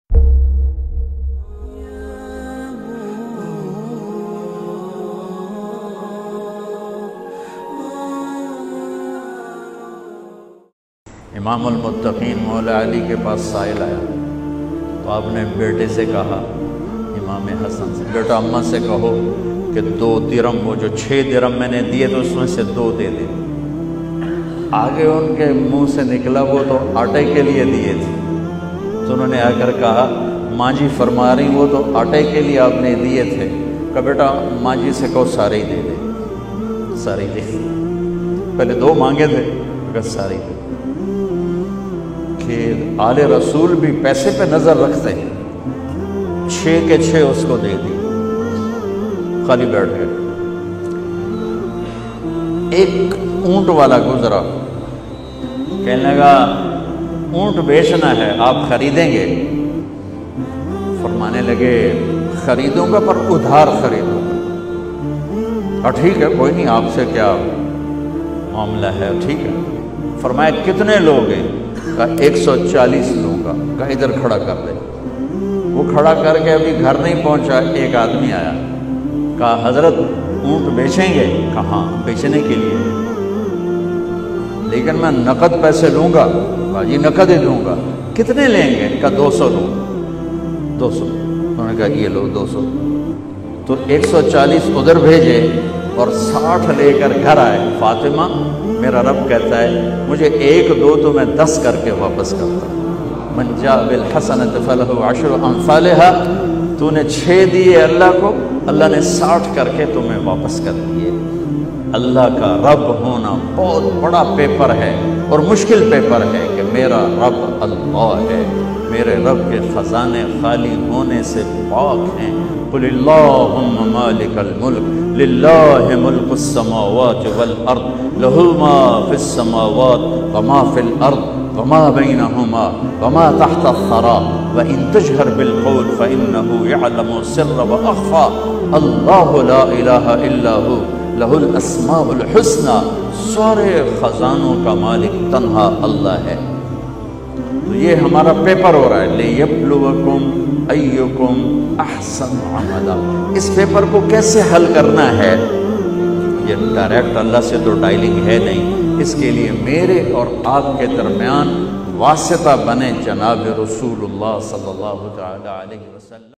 Khazano ka Malik | Hazrat Ali ( R A ) ka Waqiya | Moulana Tariq Jameel Sahab
Khazano ka Malik | Hazrat Ali ( R A ) ki Tijarat ka  Waqiya | Moulana Tariq Jameel Sahab|  | Presenting by Madrasa Zafrul Uloom |